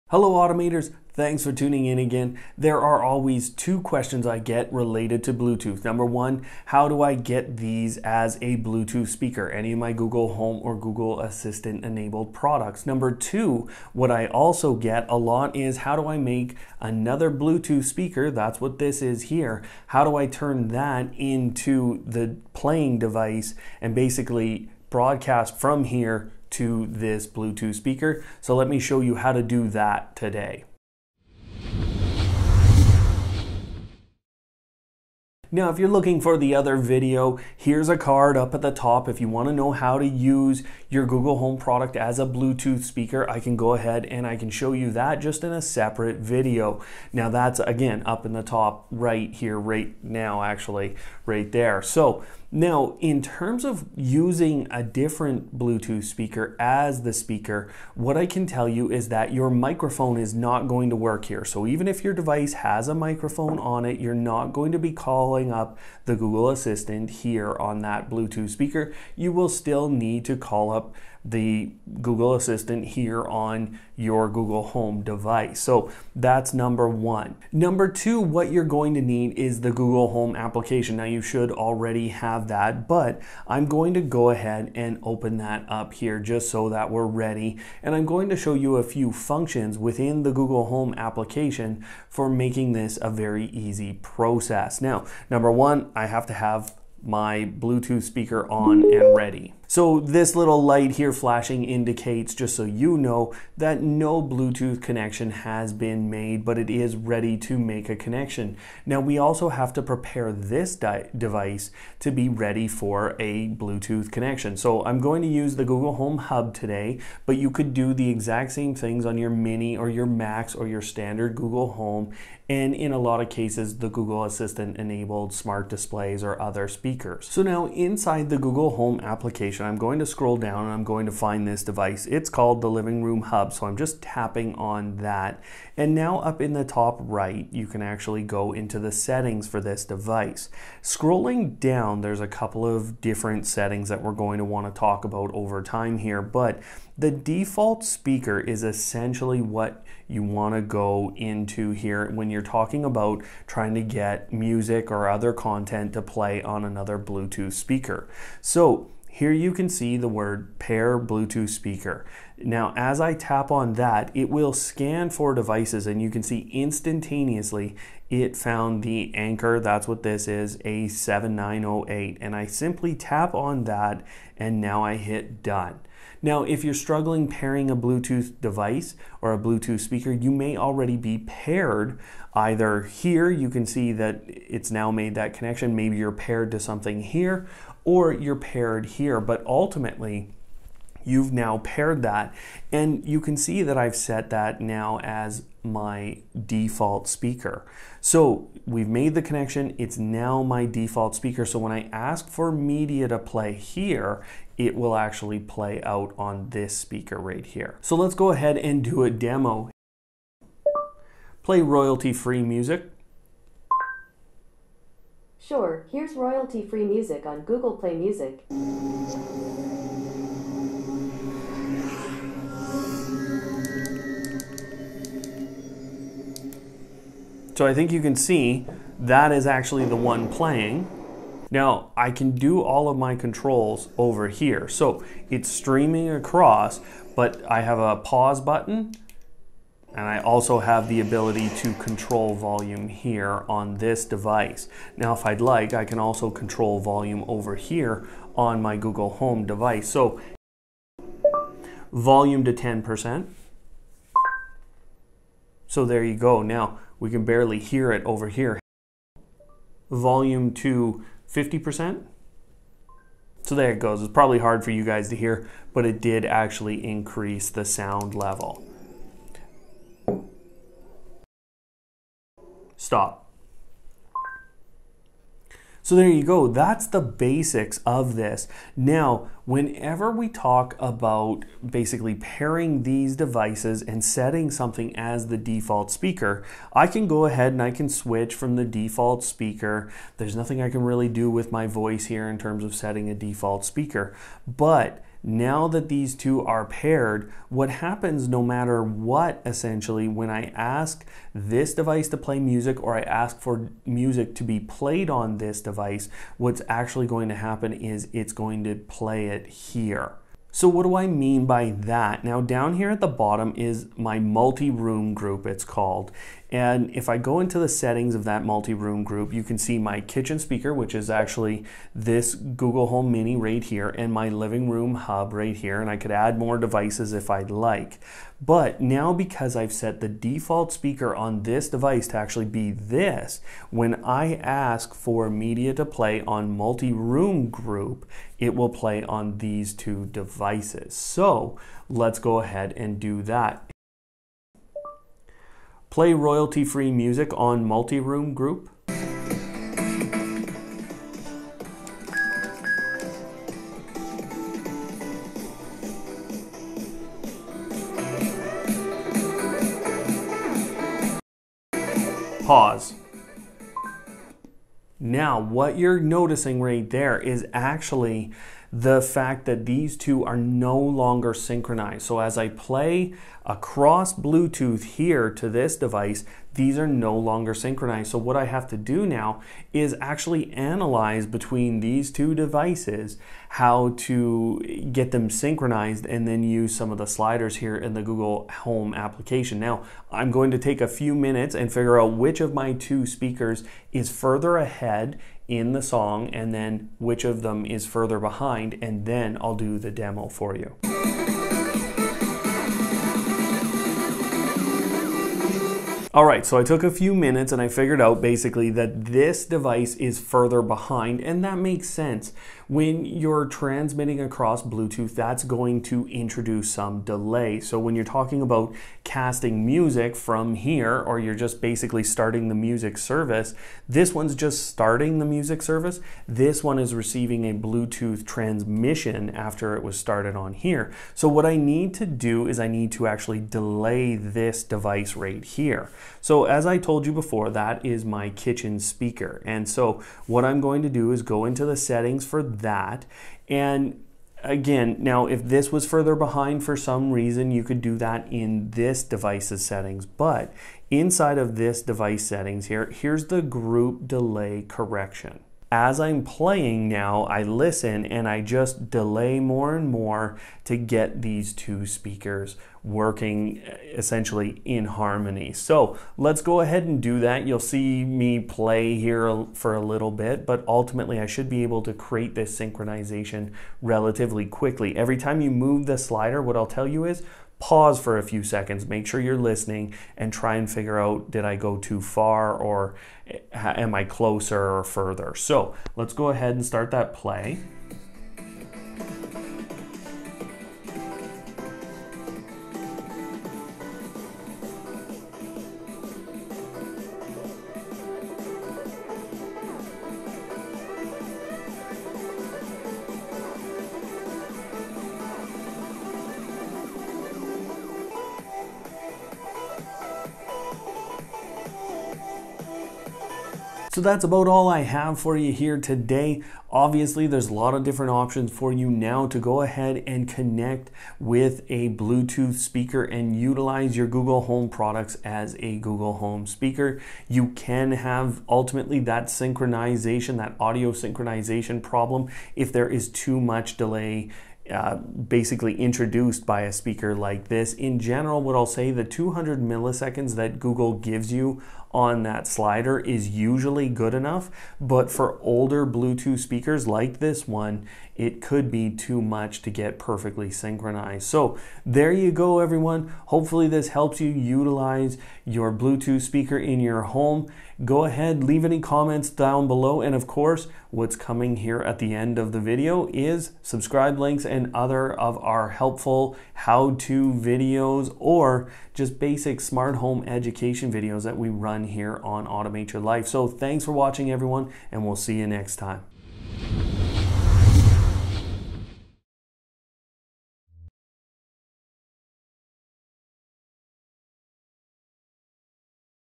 Use Your Google Home With Your Favorite Bluetooth Speaker in 2019
Pairing your Google Home, Google Home Hub, Google Home Mini, Max, or a Google Assistant-enabled device should be easy!  With our latest tutorial video, it is.  You can pair your Google Home device with your favorite Bluetooth speaker and even include it in multi-room audio sets of speakers.

One of the biggest issues right now is simply finding out how to pair your Google Home Hub or Google Home Mini with a Bluetooth speaker.  Once you find that in your device settings, you'll be able to pair easily.  Once that's happened, you can leave your Bluetooth speaker as the default speaker and therefore have it always play music and media on that Bluetooth speaker.

What's more is when you include that Google Home device in a multi-room speaker group, then media will also play on your Bluetooth speaker.

The problem with that is the introduction of a delay. Your GoogleHome doesn't introduce that much delay, but your Bluetooth speaker could if it's older and doesn't process the signal very quickly. What you'll need to do there is play with some of the synchronization settings inside your other speakers.

In the end, this video should help you get to a spot where you can use your favorite Bluetooth speaker!

Brian